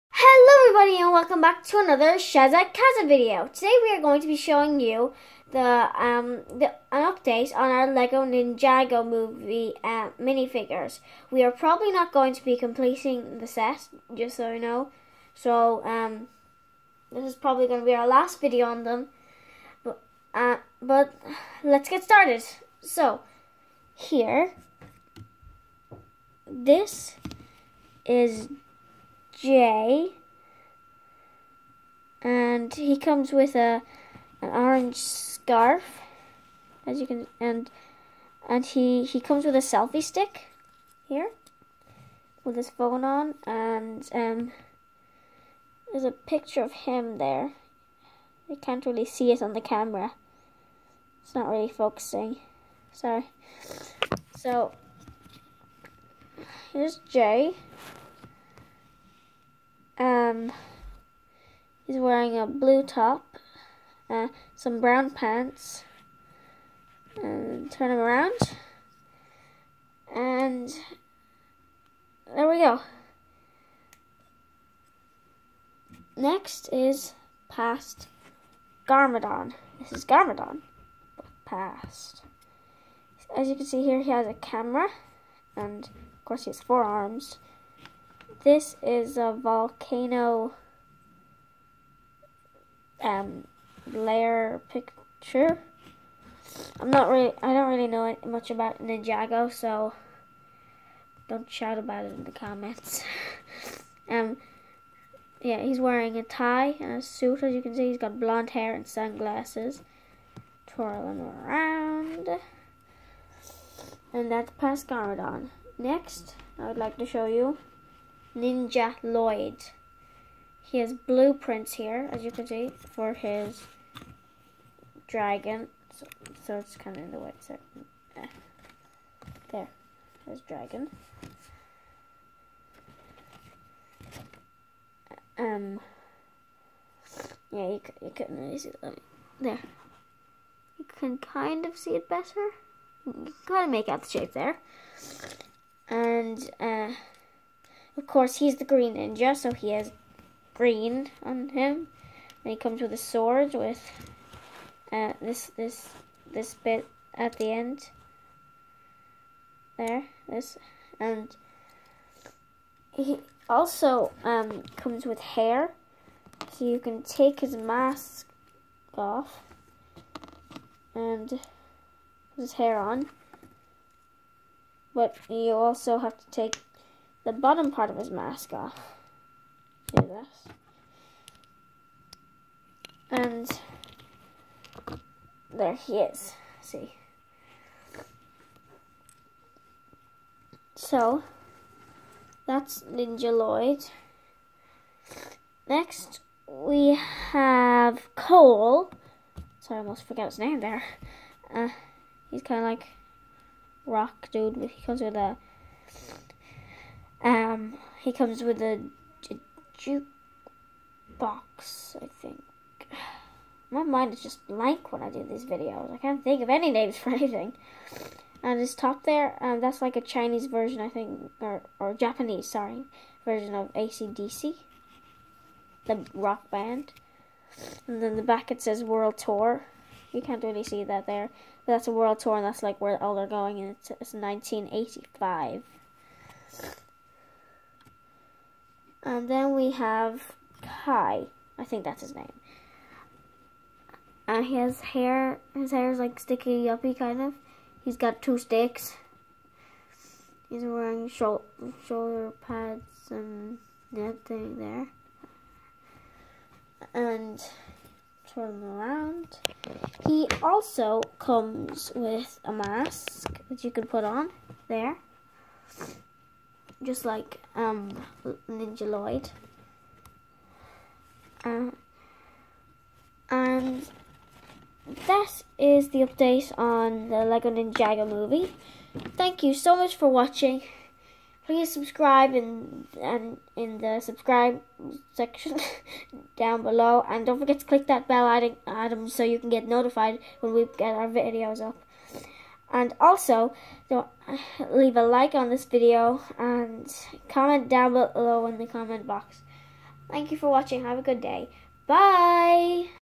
LEGO Ninjago Movie Minifigures Collection Part 2!!
In this video we show you are collection of Ninjago Movie Minifigures. It's not the entire series but we nearly have all of them!!

Remember To watch: "LEGO Movie 2 Minifigures - 10 Pack Opening!!"